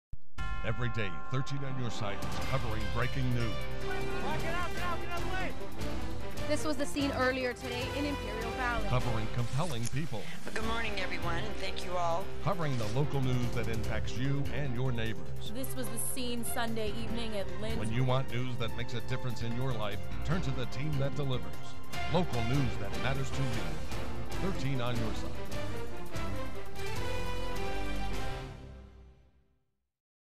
KSWT11380REV Local News Matters 30
News Promo built by Jerry Rosner in 2014 for KSWT Channel 13, 13 On Your Side